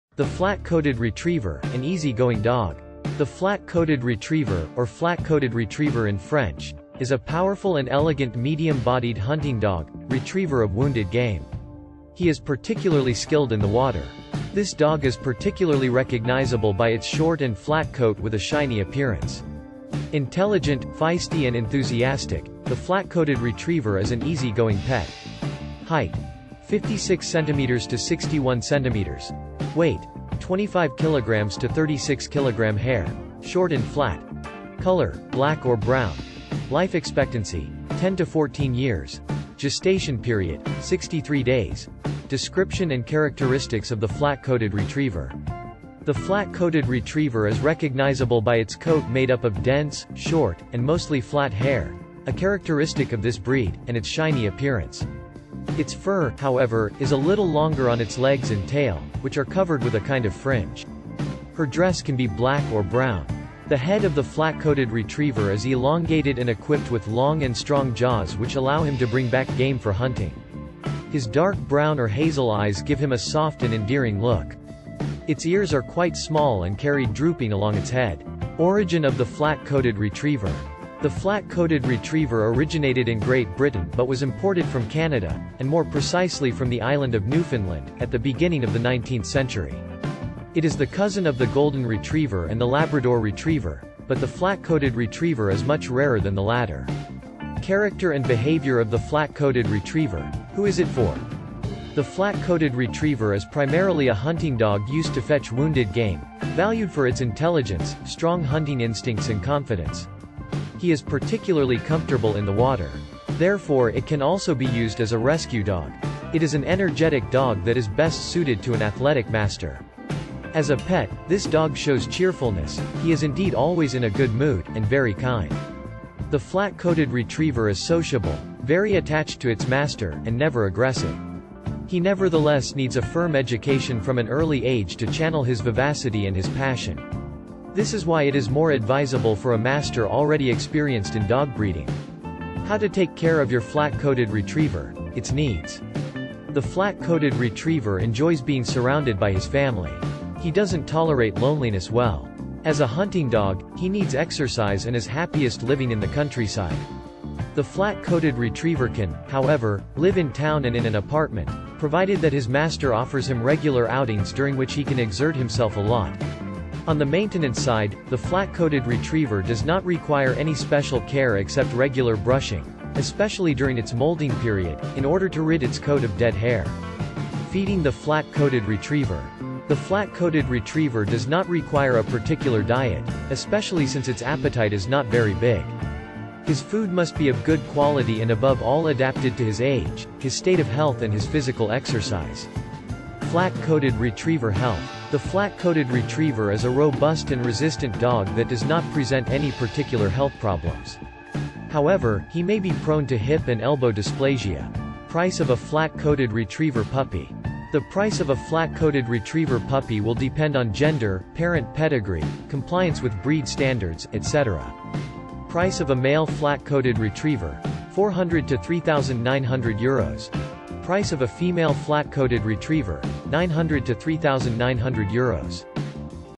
The Flat-Coated Retriever, an easy-going dog.
The Flat-Coated Retriever, an easy-going dog. The Flat-Coated Retriever, or Flat-Coated Retriever in French, is a powerful and elegant medium-bodied hunting dog, retriever of wounded game. He is particularly skilled in the water. This dog is particularly recognizable by its short and flat coat with a shiny appearance. Intelligent, feisty and enthusiastic, the Flat-Coated Retriever is an easy-going pet.

Height: 56 cm to 61 cm.
Weight: 25 kg to 36 kg.
Hair: short and flat.
Color: black or brown.
Life expectancy: 10 to 14 years.
Gestation period: 63 days.
Description and characteristics of the Flat-Coated Retriever.

The Flat-Coated Retriever is recognizable by its coat made up of dense, short, and mostly flat hair (a characteristic of this breed) and its shiny appearance. Its fur, however, is a little longer on its legs and tail, which are covered with a kind of fringe. Her dress can be black or brown.

The head of the Flat-Coated Retriever is elongated and equipped with long and strong jaws which allow him to bring back game for hunting. His dark brown or hazel eyes give him a soft and endearing look. Its ears are quite small and carried drooping along its head.

Origin of the Flat-Coated Retriever.

The Flat-Coated Retriever originated in Great Britain but was imported from Canada, and more precisely from the island of Newfoundland, at the beginning of the 19th century. It is the cousin of the Golden-Retriever and the Labrador-Retriever , but the Flat-Coated Retriever is much rarer than the latter.

Character and behavior of the Flat-Coated Retriever – Who is it for?

The Flat-Coated Retriever is primarily a hunting dog used to fetch wounded game, valued for its intelligence, strong hunting instincts and confidence. He is particularly comfortable in the water. Therefore it can also be used as a rescue dog. It is an energetic dog that is best suited to an athletic master.

As a pet, this dog shows cheerfulness, he is indeed always in a good mood, and very kind. The Flat-Coated Retriever is sociable, very attached to its master, and never aggressive. He nevertheless needs a firm education from an early age to channel his vivacity and his passion. This is why it is more advisable for a master already experienced in dog breeding.

How to take care of your Flat-Coated Retriever: its needs.

The Flat-Coated Retriever enjoys being surrounded by his family. He doesn’t tolerate loneliness well.

As a hunting dog, he needs exercise and is happiest living in the countryside. The Flat-Coated Retriever can, however, live in town and in an apartment, provided that his master offers him regular outings during which he can exert himself a lot.

On the maintenance side, the Flat-Coated Retriever does not require any special care except regular brushing, especially during its moulting period, in order to rid its coat of dead hair.

Feeding the Flat-Coated Retriever.

The Flat-Coated Retriever does not require a particular diet, especially since its appetite is not very big. His food must be of good quality and above all adapted to his age, his state of health and his physical exercise.

Flat-Coated Retriever Health.

The Flat-Coated Retriever is a robust and resistant dog that does not present any particular health problems. However, he may be prone to hip and elbow dysplasia.

Price of a Flat-Coated Retriever Puppy.

The price of a Flat-Coated Retriever puppy will depend on gender, parent pedigree, compliance with breed standards, etc.

Price of a male Flat-Coated Retriever: 400 to 3,900 €.
Price of a female Flat-Coated Retriever: 900 to 3,900 €.